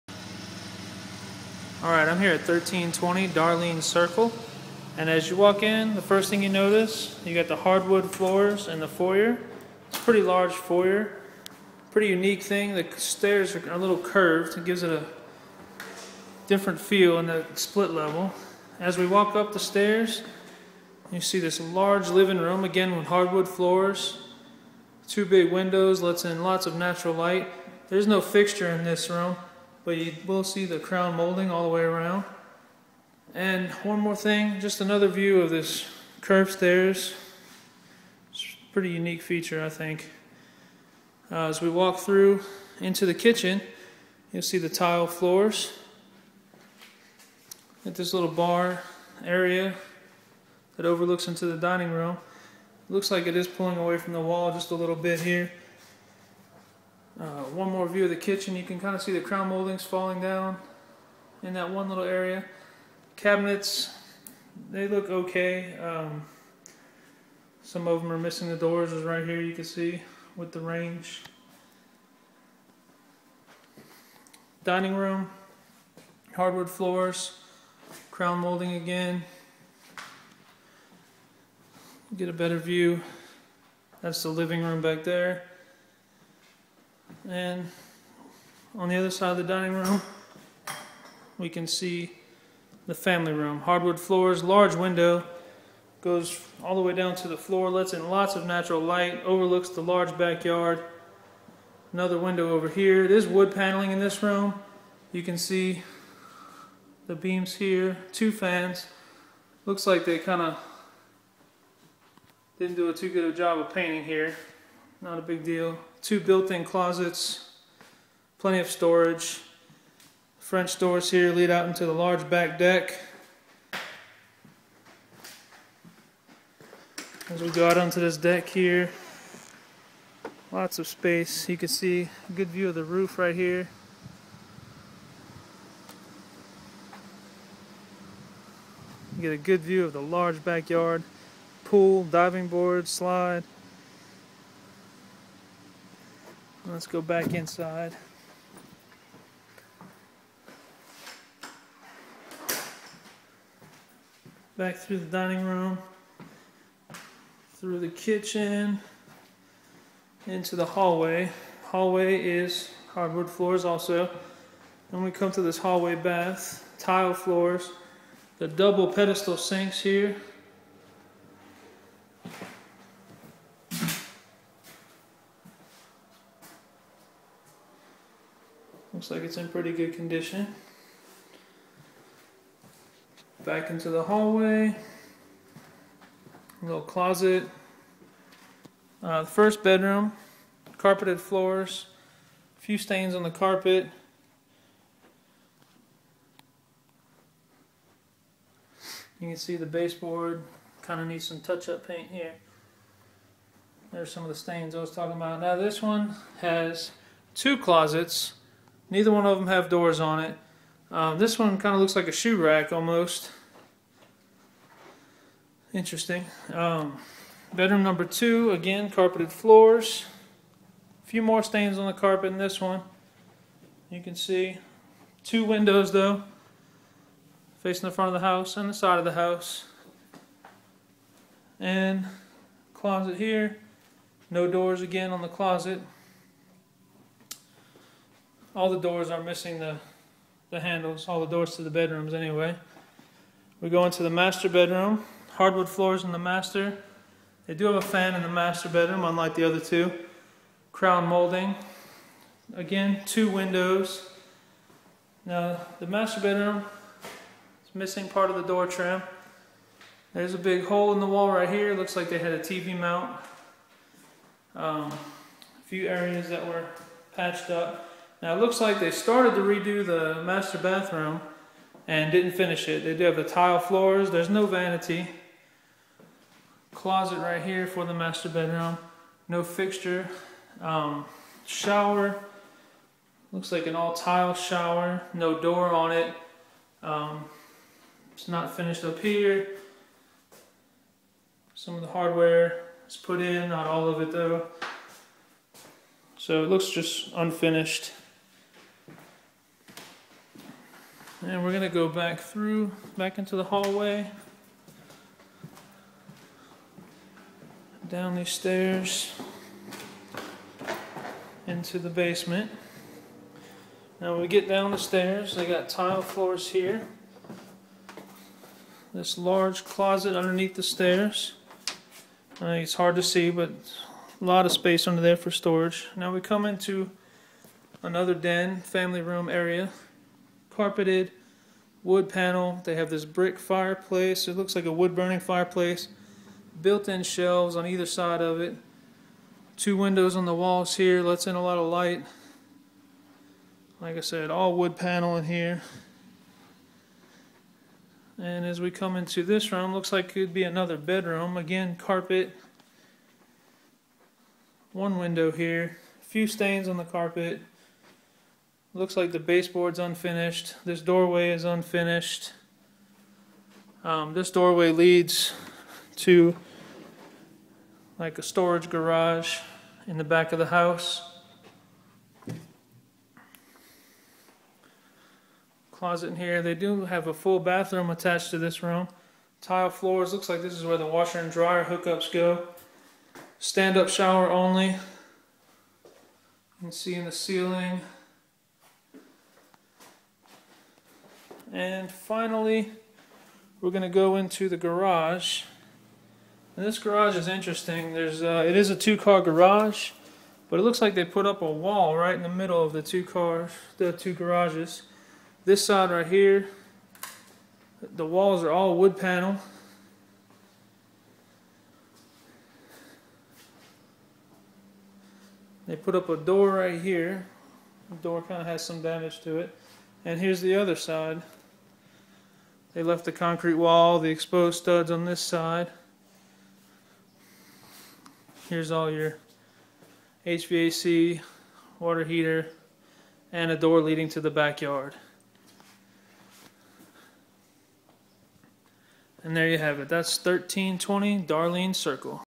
1320 Darlene Circle Chattanooga TN 37412 HUD owned home for sale
1320 Darlene Circle is a 4 bedroom 3 bathroom HUD owned home for sale in Chattanooga TN 37412